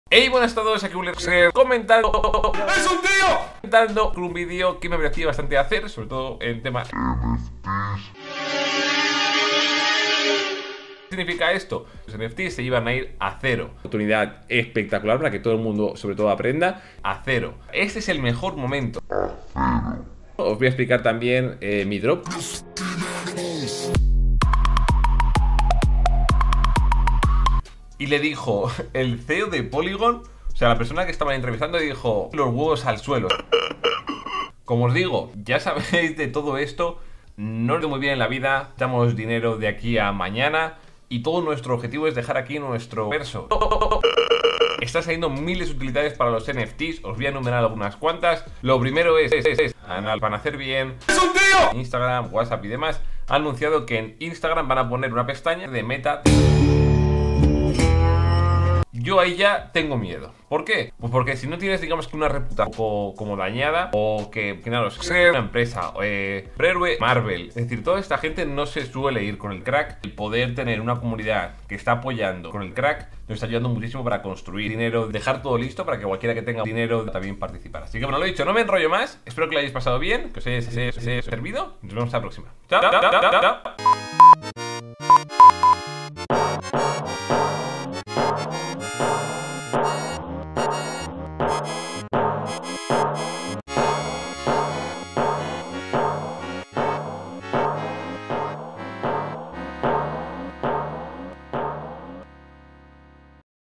[YTPH] Willyrex NECESITA DINERO, compren sus NFT
⚠Parodia del video "POR QUÉ TUS NFTS, y los mios, SE FUERON A 0$" versión YTPH⚠

Si te ha gustado el vídeo, te dejo unos links para que sigas apoyando💪.